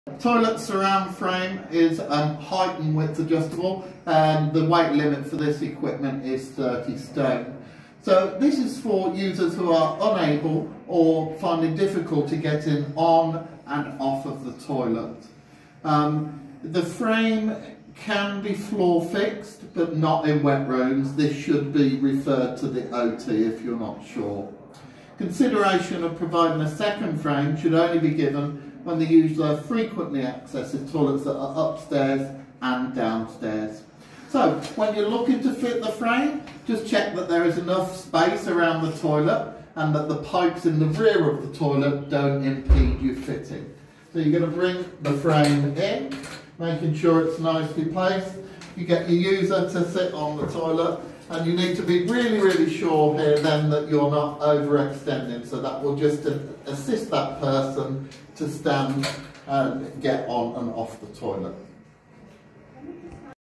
Toilet Surround Frame
This video should be watched as part of the online Low Level Equipmemt Prescriber Training